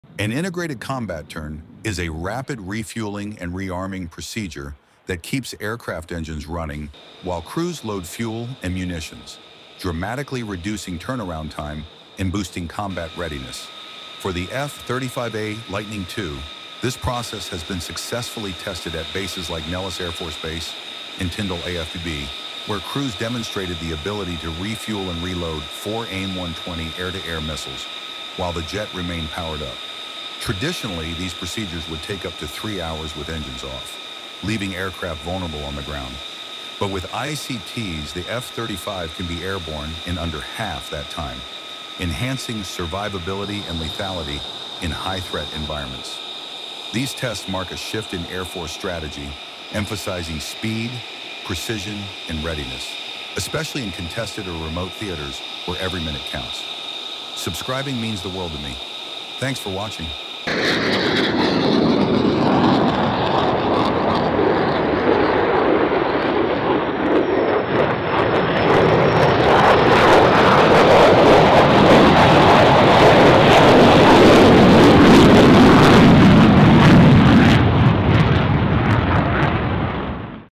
F-35A Lightning II: Rapid Combat Turnarounds
Discover the next level of combat readiness with the F-35A Lightning II! Join us as teams from Nellis and Tyndall AFB unveil the cutting-edge Integrated Combat Turn (ICT), a method that significantly reduces refueling and re-arming times. Witness the incredible efficiency as crews keep the engines running while swiftly reloading fuel and AIM-120 missiles, allowing the jet to be mission-ready in less than half the usual time. This advancement not only boosts operational effectiveness but also reduces vulnerability on the ground. Explore how this pivotal change in Air Force strategy is reshaping air combat with speed and precision in contested and remote theaters. Your support is invaluable as we delve into these remarkable innovations.